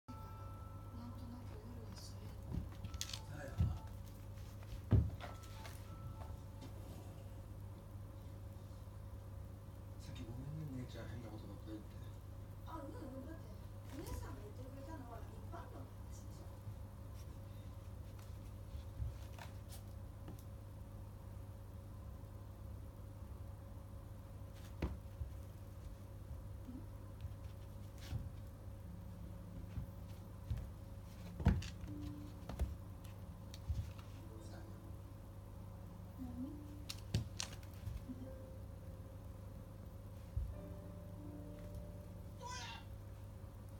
Cat playing with gray ball
She is playing with caster toy with gray ball.